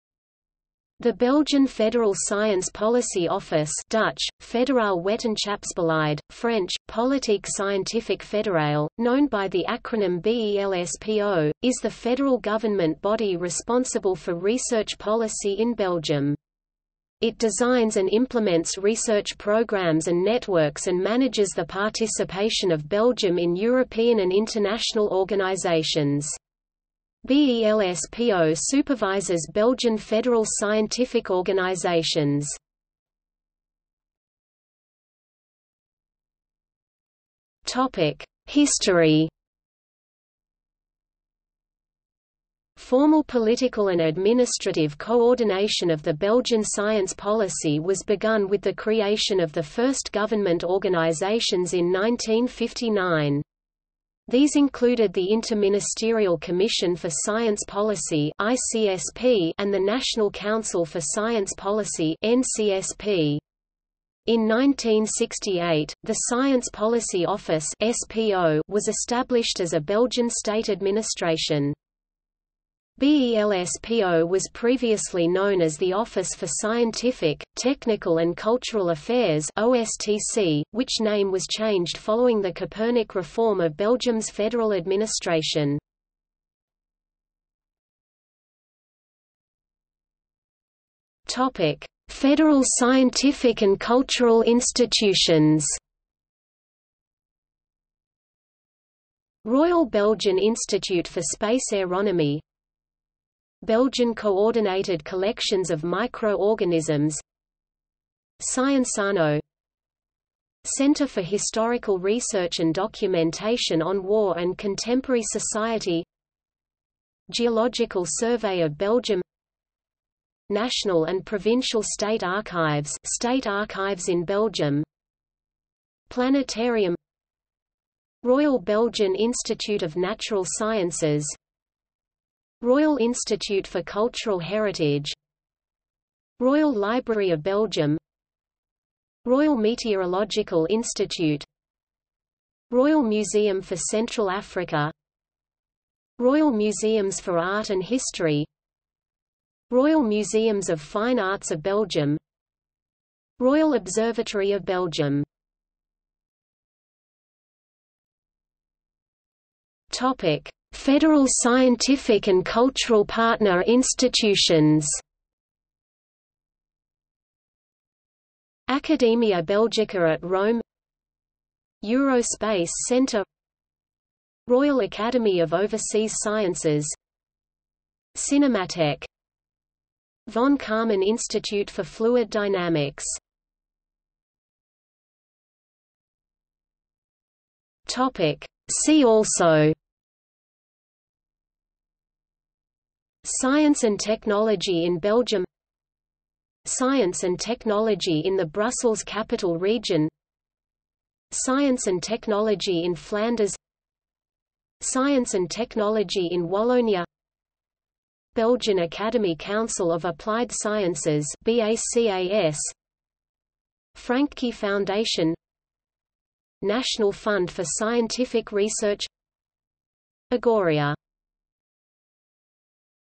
Belgian Federal Science Policy Office | Wikipedia audio article
00:00:34 1 History
00:01:27 2 Federal scientific and cultural institutions
00:02:36 3 Federal scientific and cultural partner institutions
00:03:04 4 See also

- Socrates

SUMMARY
The Belgian Federal Science Policy Office (Dutch: Federaal Wetenschapsbeleid; French: Politique scientifique fédérale), known by the acronym BELSPO, is the federal government body responsible for research policy in Belgium. It designs and implements research programmes and networks and manages the participation of Belgium in European and international organisations. BELSPO supervises Belgian federal scientific organisations.